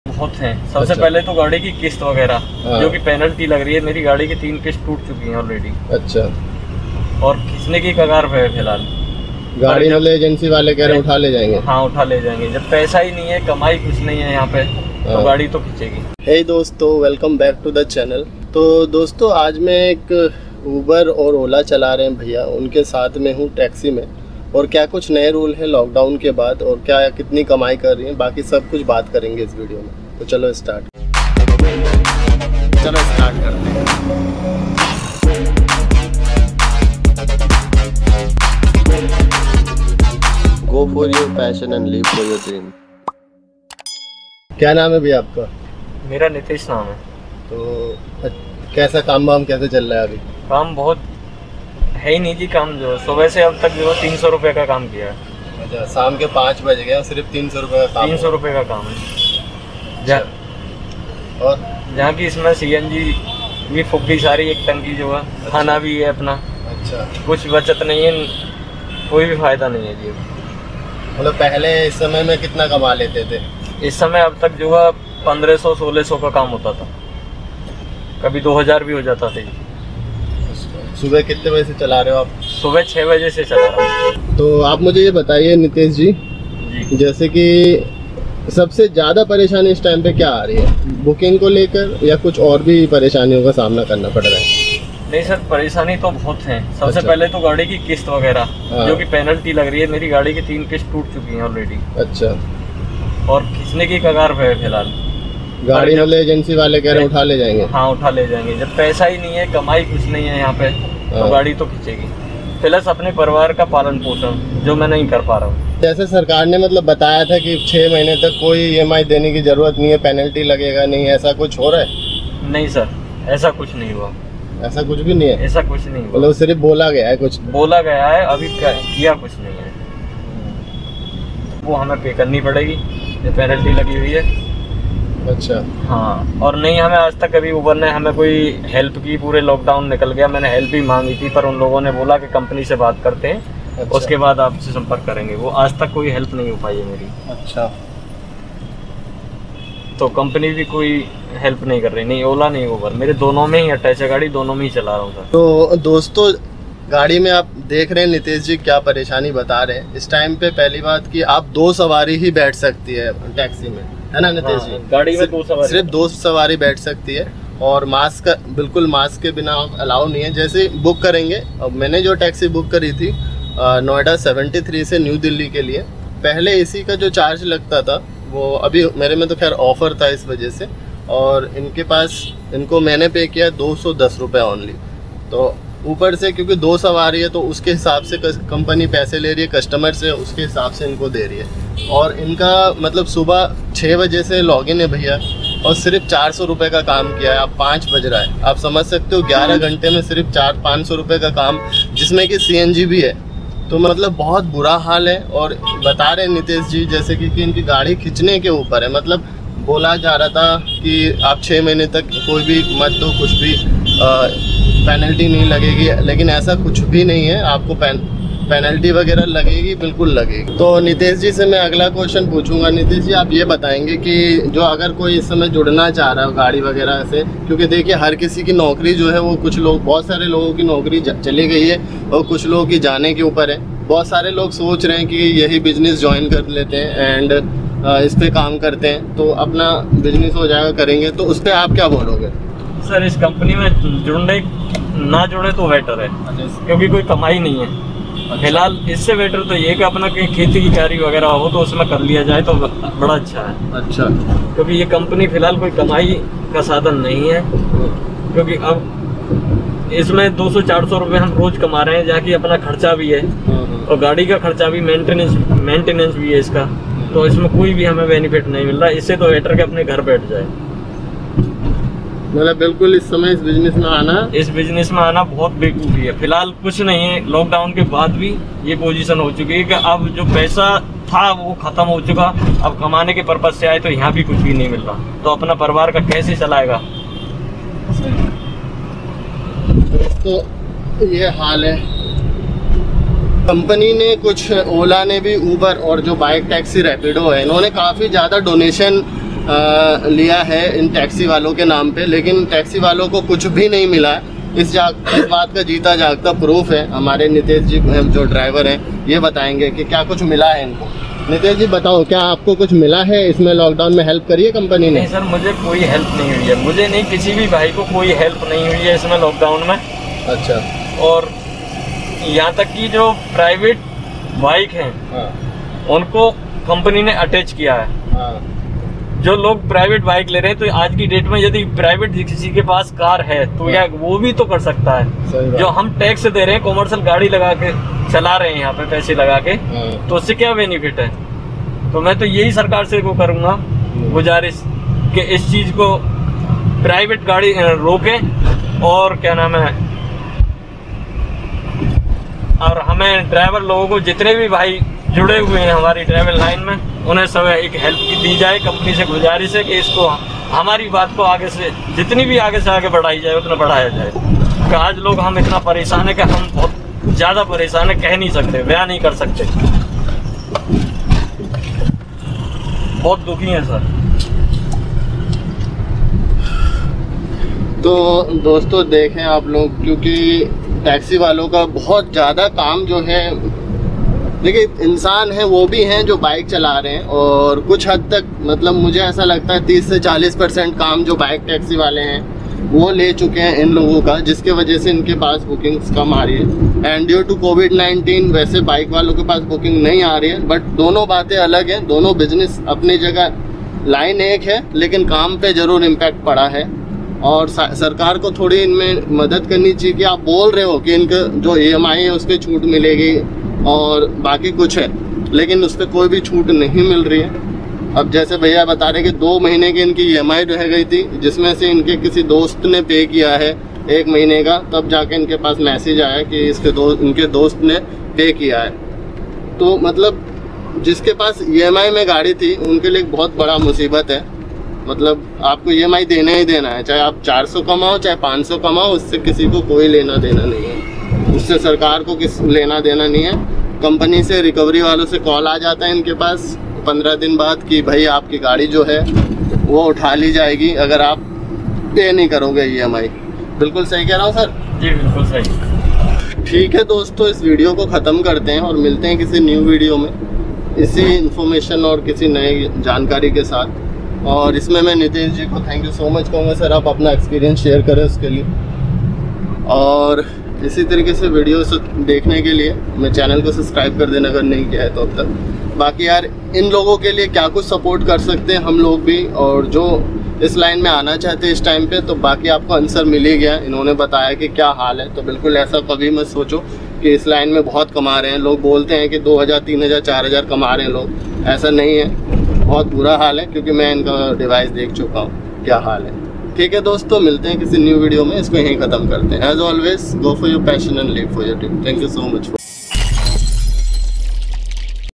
Ola Uber Cab Driver in Bad Situation after Lockdown
Hey Dosto welcome back to another video
It's me Vishwanath Dey Your Rapido Captain

In this video we are talking about Uber cab business after lockdown